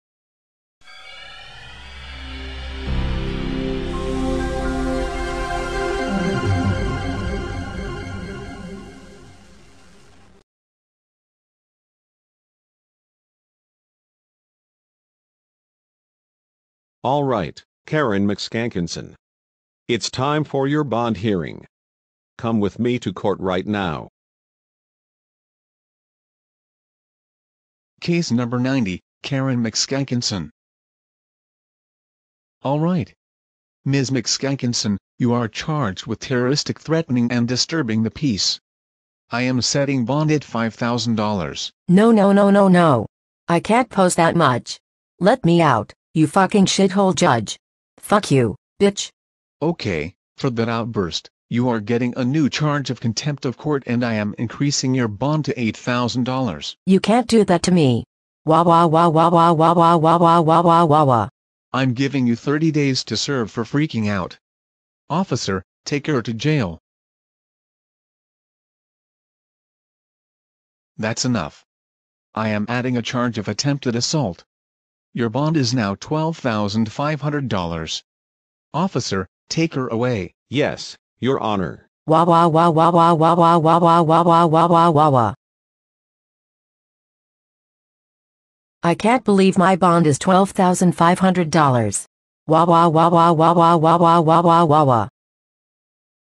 Karen McSkankinson's Bond Hearing (Super Late)
Sorry this was late. School work is really heavy and I have little to no time to make these. Karen is mad at judge Ming Wenhao for sure!